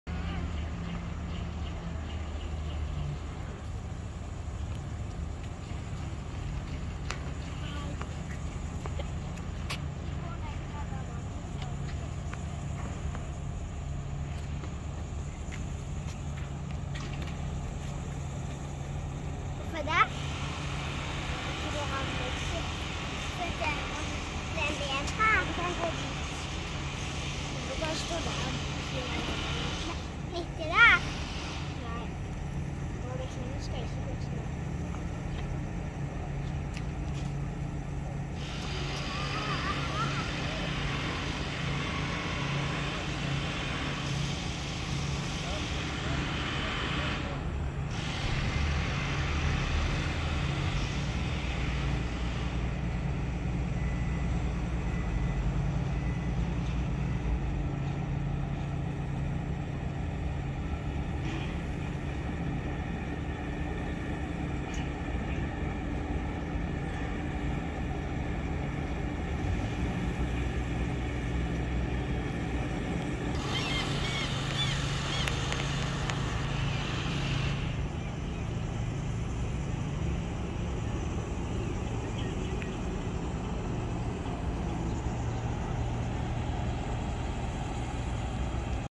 25/07/21 M/F Karlsøyfjord (Boreal) leaving Molde | Norway
M/F Karlsøyfjord (operated by Boreal) leaving Molde bound for Vestnes on a sunny day in northwestern Norway

M/F Karlsøyfjord (driftes av Boreal) forlater Molde mot Vestnes på en solrik dag i Møre og Romsdal